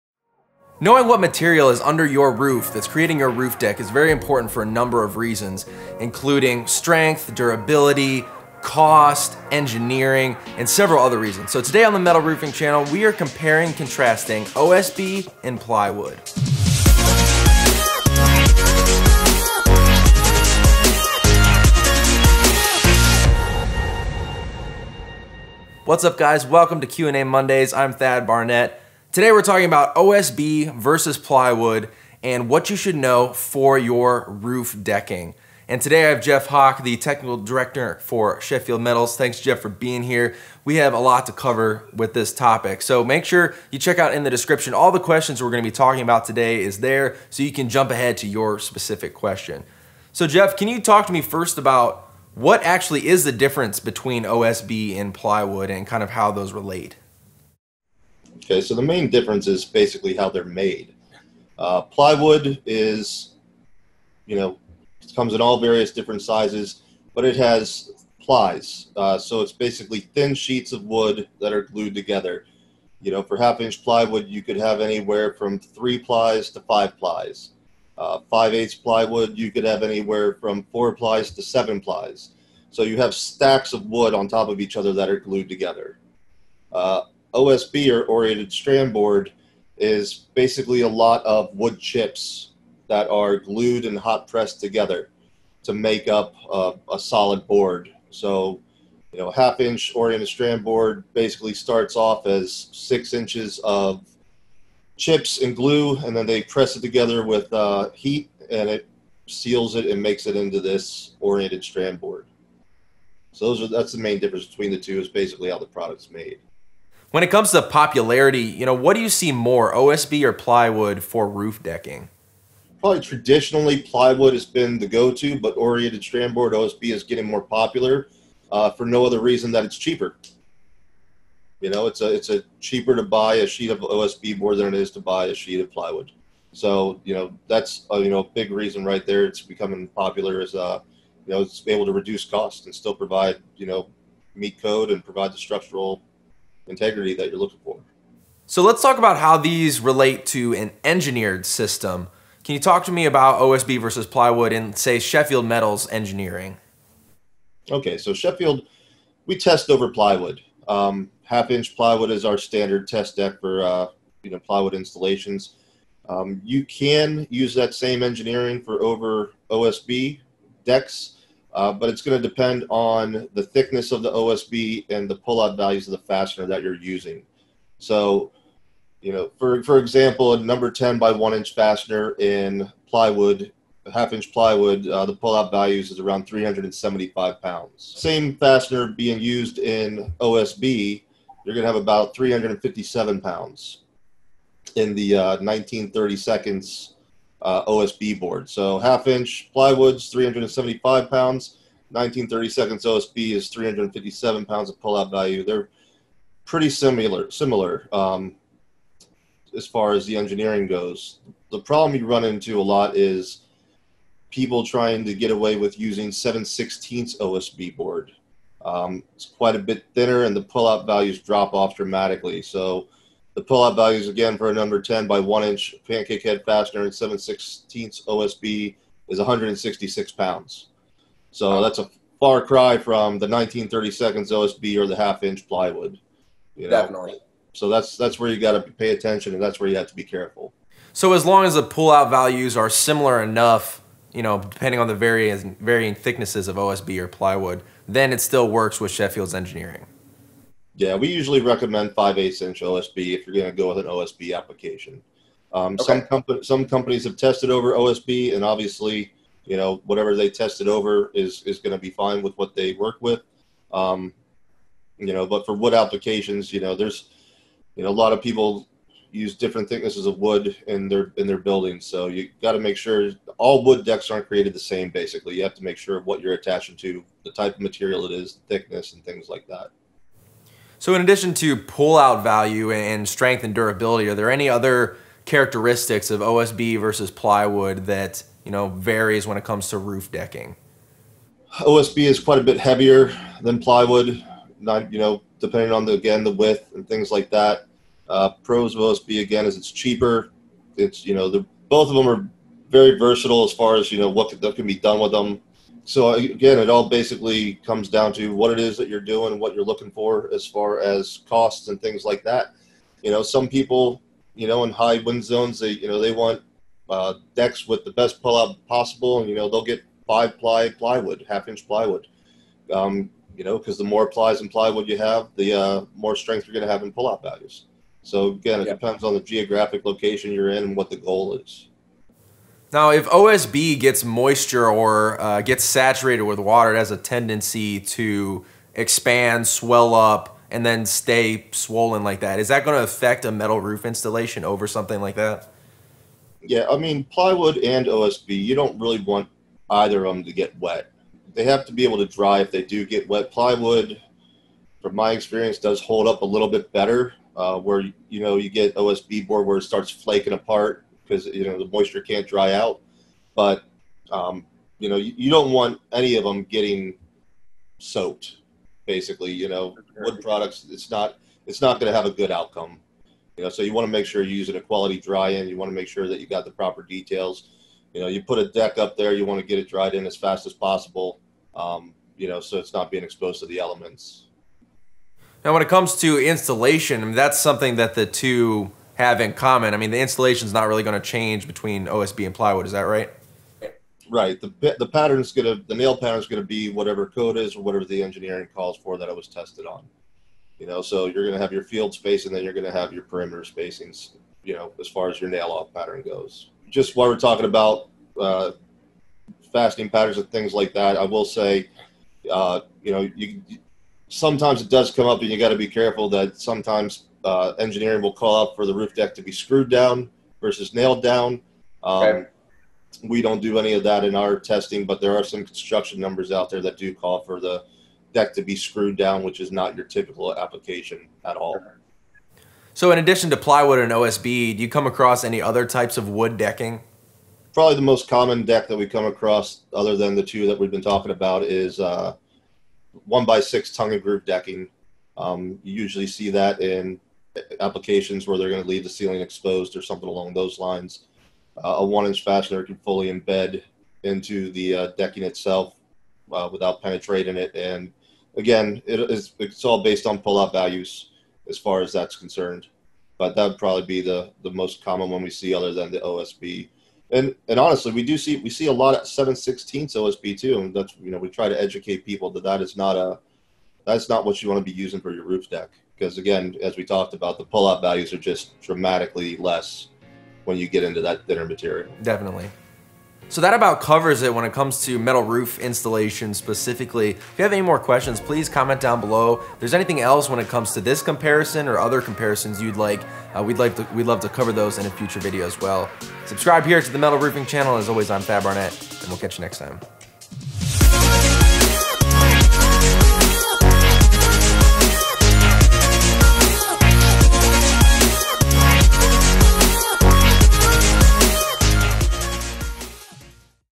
OSB vs. Plywood: Which Should You Choose for Your Roof Deck?
What do you need to know when deciding between OSB and plywood for your roof deck?

Oriented strand board (OSB) and plywood are both popular choices for roof decking, but what are the differences? On today's Q&A Mondays episode, Jeff Hock joins Thad to discuss differences in price, strength, durability, and overall performance between the two. They also cover how material thickness affects pull-out value of the system's fasteners as well as additional engineering considerations.

Topics covered:
►Intro 00:00
►What is the difference between OSB and plywood? 0:53
►Is OSB or plywood more popular for roof decking? 1:59
►How do OSB and plywood relate to Sheffield Metals' engineering and testing? 2:33
►What characteristics of each type matter when it comes to roof decking? 4:48
►How does water saturation affect each type? 5:41
►Is installation different for OSB and plywood? 6:29
►Are there any other types of wood decking? 7:14